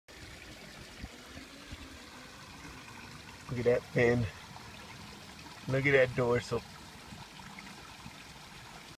The Joy Of Koi - "Marlin"
I have a hard time when I see a fish with unique characteristics that I don't have in my pond. I call this little fella Marlin because of his yellow sail fin dorsal.  Of course I didn't even go to the store looking for fish - I went to buy a skimmer for my new turtle pond ;-)